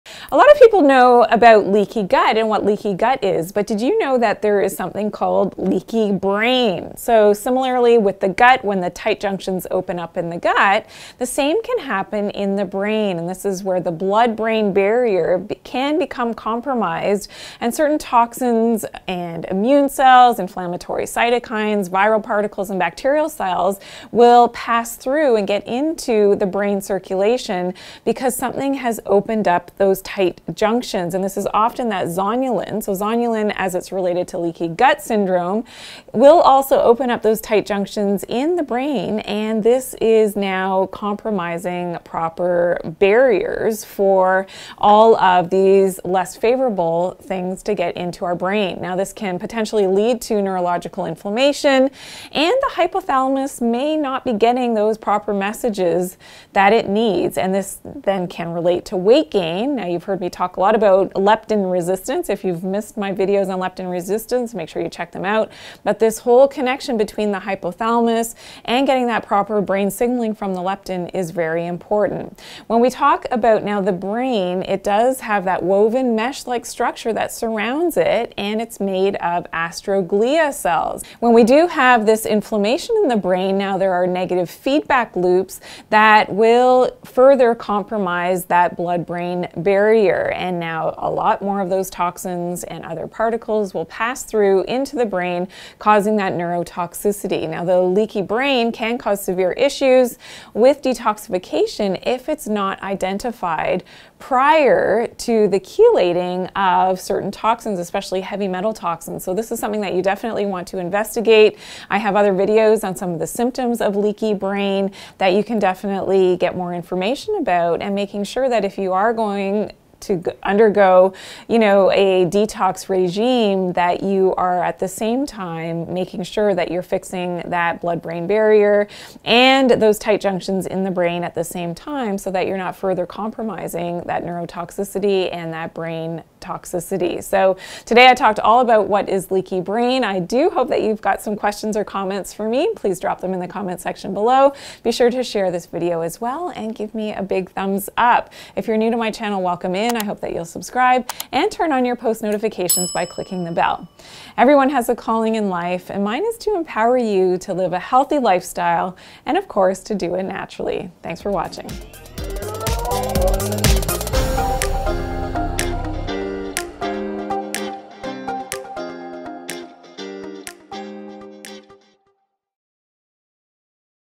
Brain Detox | What is Leaky Brain | Dr. J9 Live
In this video, Dr. Janine explains what is leaky brain.  She looks at how, like the gut, it happens when the tight junctions in the brain become compromised allowing toxins into the bloodstream.  She looks at how a leaky brain can lead to things like leptin resistance and hormonal imbalances.  She talks about how a leaky brain can also lead to brain inflammation.  Lastly, Dr. Janine explains how a leaky brain can cause severe issues with detoxification.

Video Chapters
00:00 – Intro
00:10 – Tight Junctions in the Brain
00:35 – Zonulin Contributes to Opening Tight Junctions
00:48 – Can Lead to Neurological Inflammation
01:00 – Can Contribute to Leptin Resistance
01:22 – Inflammation can Lead to Negative Feedback Loops
01:40 – Severe Detoxification Issues